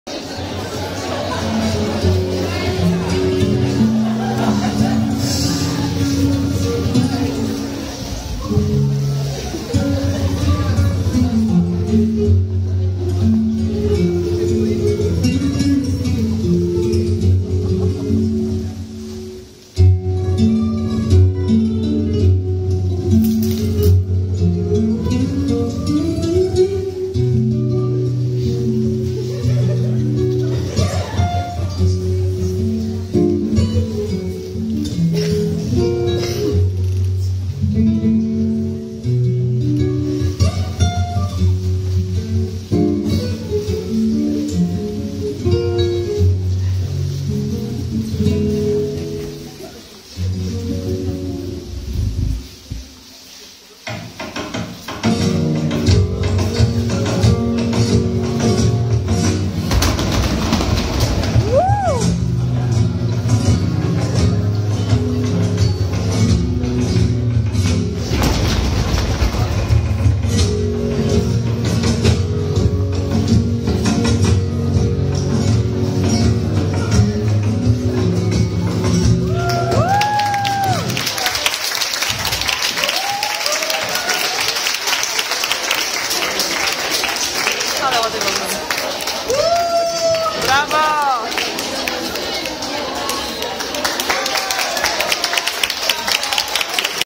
Dzień hiszpański - 1D
Oto płonące flamenco klasy 1D.

Zdjęcia i montaż: Karolina Cichocka